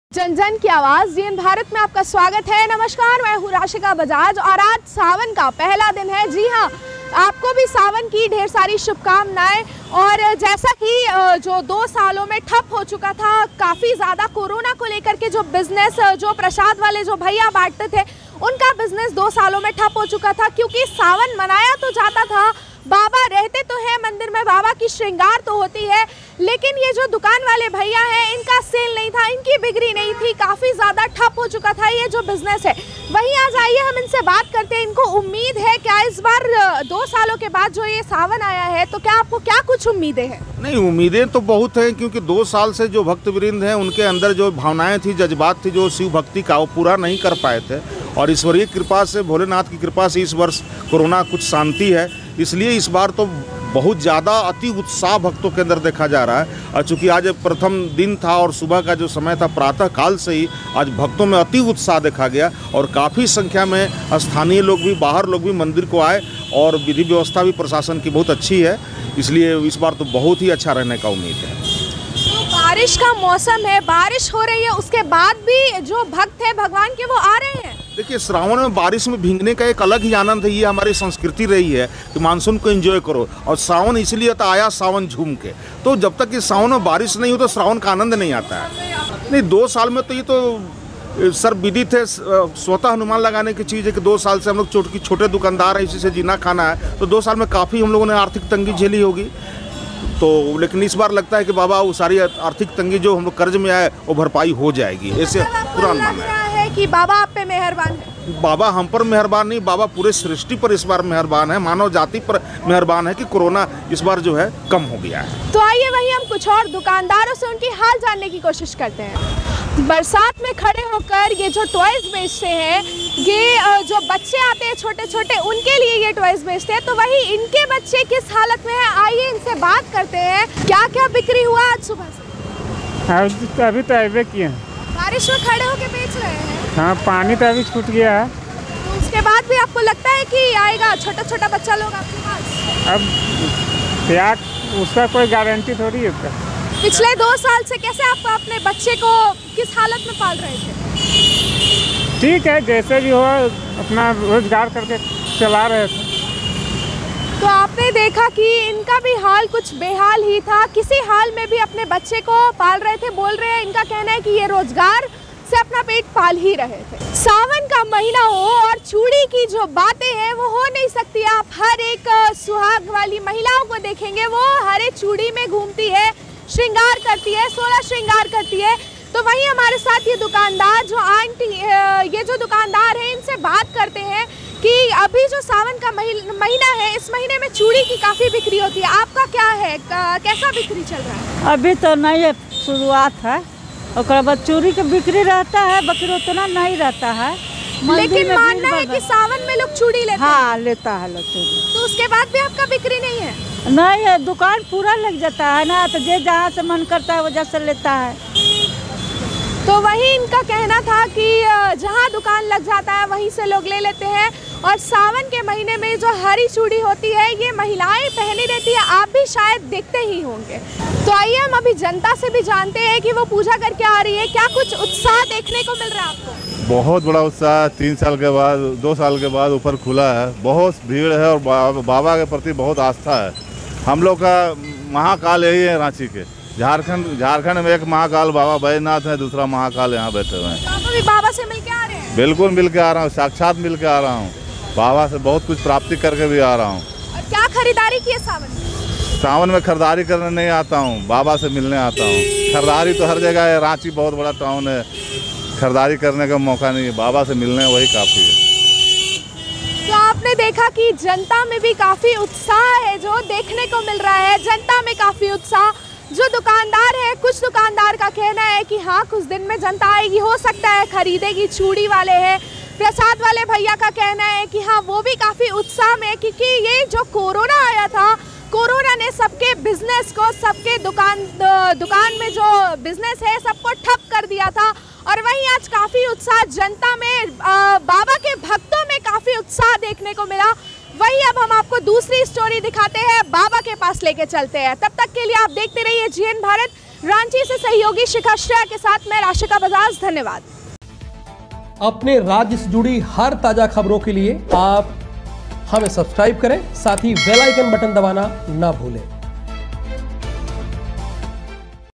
सावन के आगाज के साथ ही दुकानदारों के चेहरे पर भी लौटी रौनक || Ranchi News || Sawan Mahina
सावन के आगाज के साथ ही दुकानदारों के चेहरे पर भी लौटी रौनक